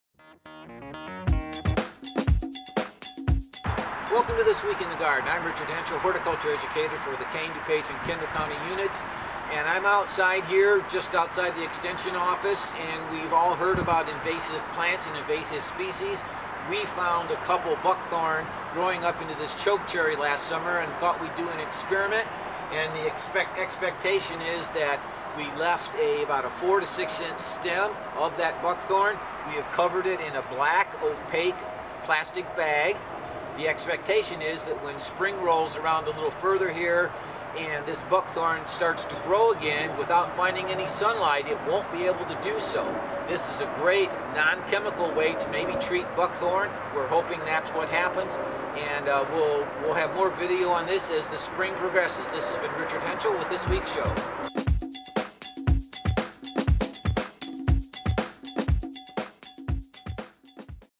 This Week in the Garden - Buckthorn pt1
Buckthorn is an invasive species and U of I Extension Educator Richard Hentschel talks about testing one method to get rid of the pest. May 2017.